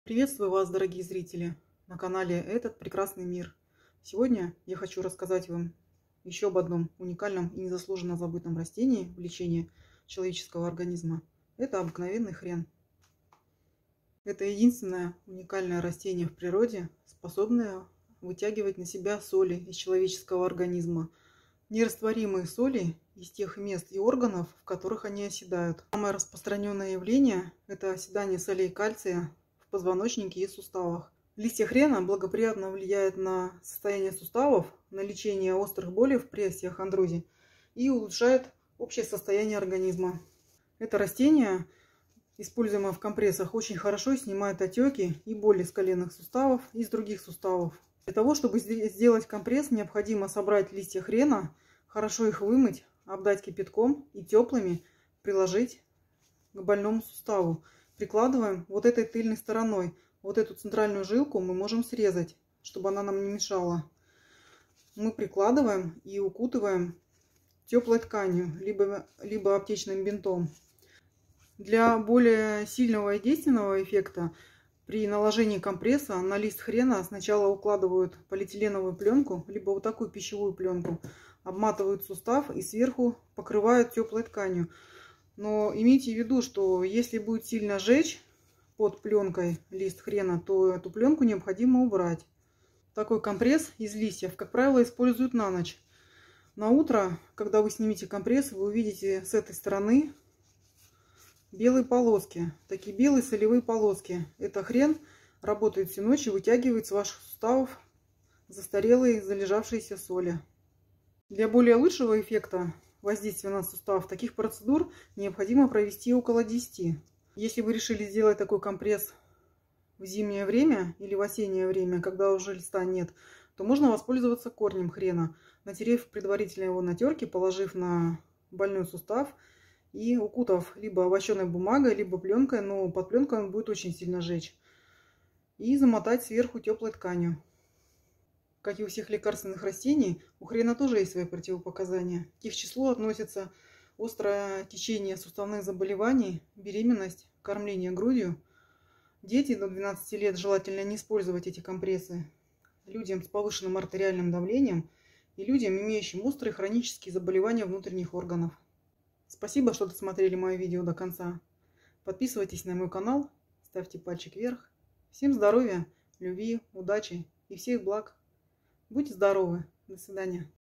Компресс из листа хрена- спасение от остеохондроза и болей в суставах!!!
Хрен-единственное растение, способное выводить через кожу соли из организма.  Компресс из листа хрена хорошо снимает отёки с суставов, уменьшает дискомфорт и болезненные ощущения в суставах,выводит токсины из организма и очищенают кровь,снижает отечностьв проблемных участках, укрепляет  и  активирует рост хрящевых клеток;
Лист хрена тщательно помыть, облить кипятком и приложить в больному суставу, сверху уложить плёнку (при необходимости), и указать тёплой тканью. Оставить на ночь. Если будет сильное жжение,  компресс необходимо снять.